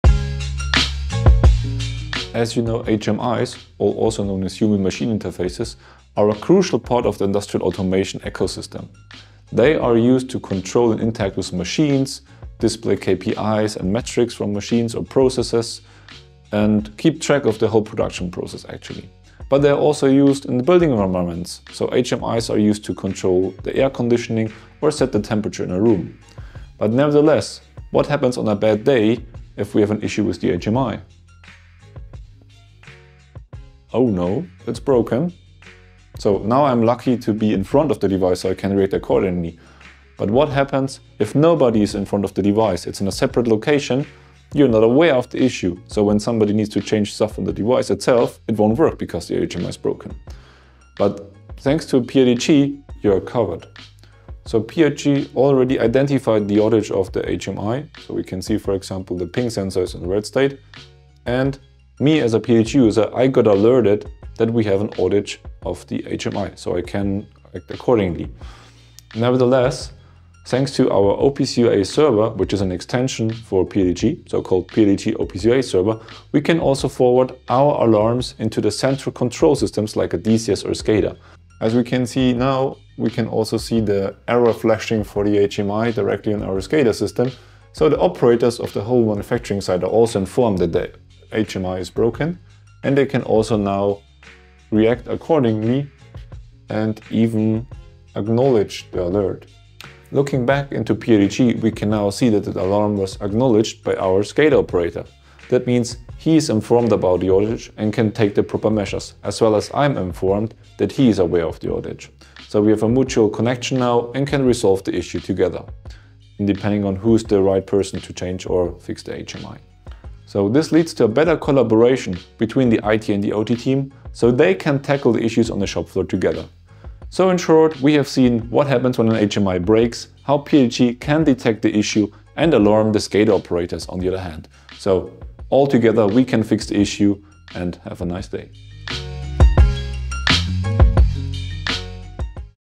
HMI Monitoring with the Paessler PRTG OPC UA Server
HMIs are interfaces that allow humans to interact with, monitor and control machines and industrial processes. They are a crucial part of industrial automation.

More informations 👇
Paessler PRTG monitoring software can detect HMI outages and generate alerts. Thanks to the product extension Paessler PRTG OPC UA Server, the alarm can be forwarded to a supervisory and control system such as a SCADA or DCS. There the operator is informed of the HMI failure, and the alarm can also be acknowledged.

This leads to better collaboration between IT and OT and the issue with the HMI can be tackled and fixed together.